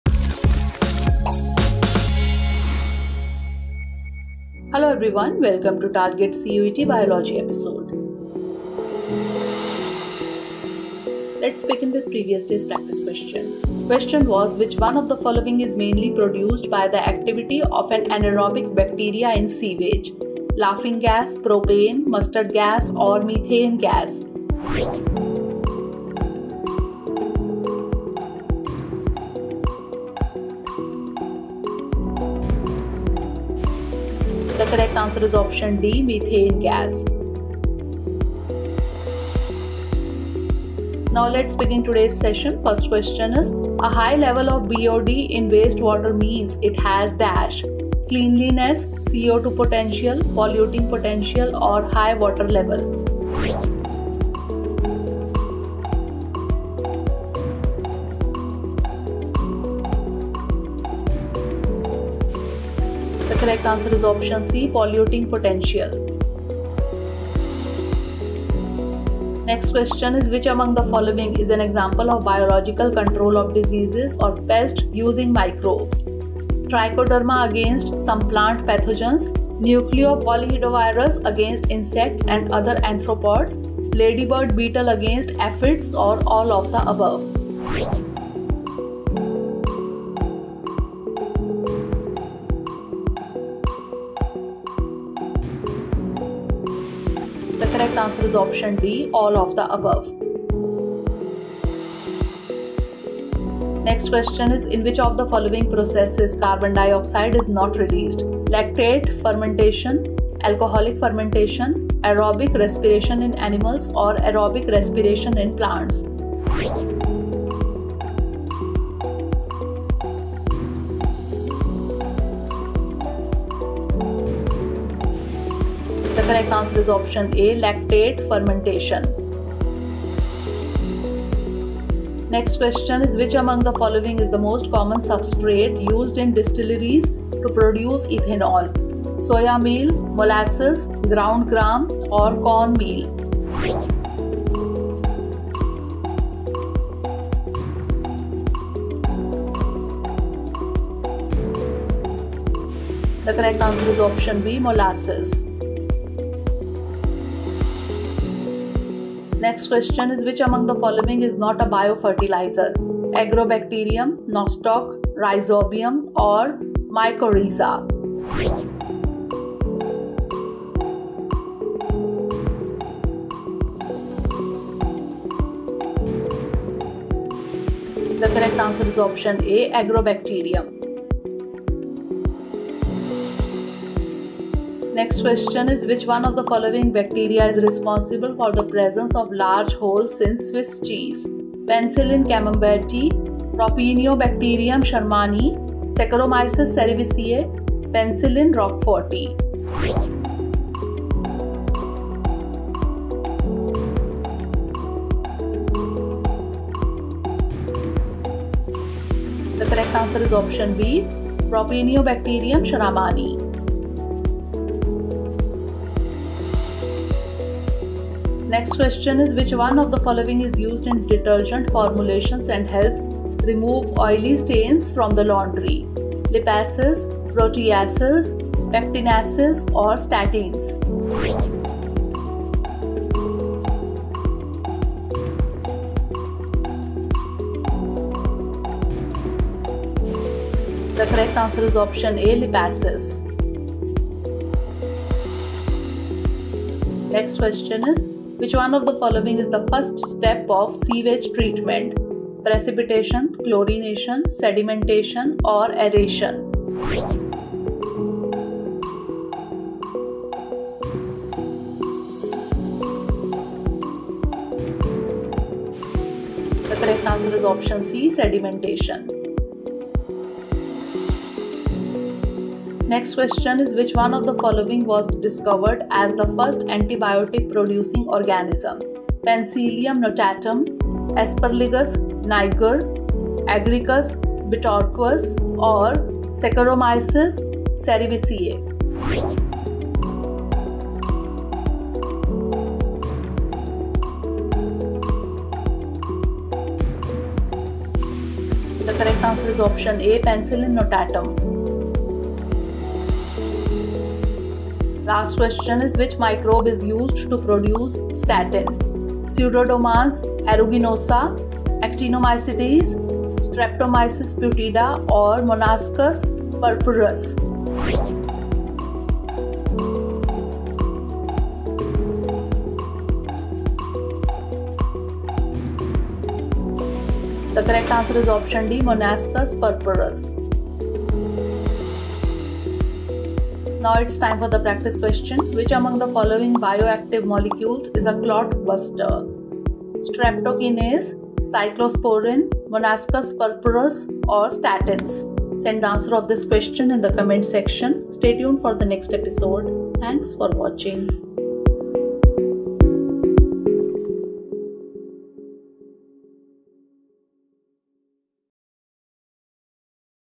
Target CUET 2023 | Biology | MCQ Test | Episode - 34 | Drishti CUET English
Dear Viewers,
Drishti CUET English brings to you the Target CUET 2023 program. Target CUET 2023 will help you to practice questions based on the most important topics for the CUET 2023 Exam. This video covers Biology MCQs from the Domain section.

1. BOD
2. Biological Control of Diseases
3. Lactate Fermentation
4. Molasses
5. Biofertilizer
6. Propionibacterium Sharmanii
7. Lipases
8. Sedimentation
9. Penicillium Notatum
10. Monascus Purpureus
11. Streptokinase